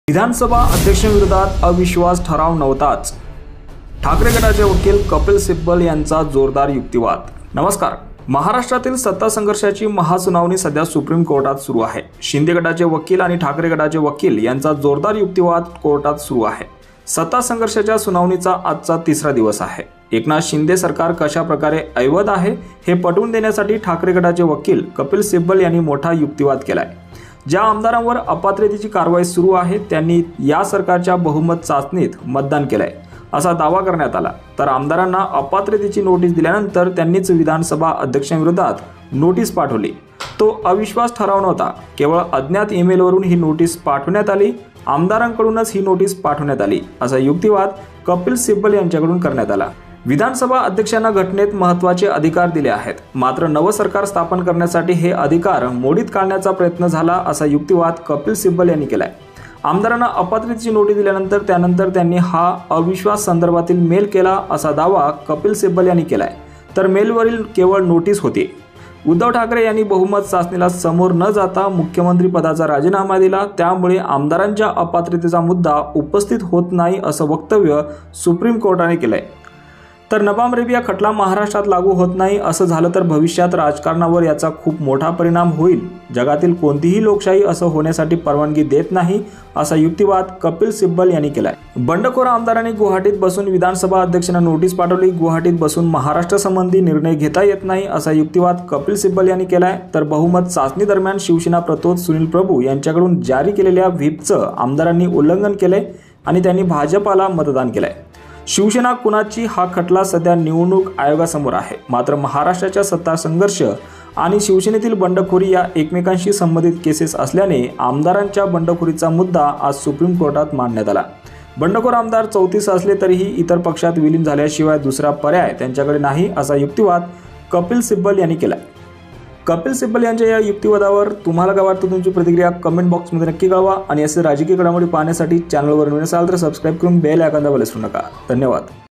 सुप्रीम कोर्टात ठाकरेंच्या वकिलांनी शिंदे गटाची केली कोंडी ! Shivsena Hearing in supreme Court
आमदारांना अपात्रतेची नोटीस दिल्यानंतर त्यांनीच विधानसभा अध्यक्षांविरोधात नोटीस पाठवली. तो अविश्वास ठराव नव्हता. केवळ अज्ञात ईमेलवरून ही नोटीस पाठवण्यात आली. आमदारांकडूनच ही नोटीस पाठवण्यात आली, असा युक्तिवाद कपिल सिब्बल यांच्याकडून करण्यात आला.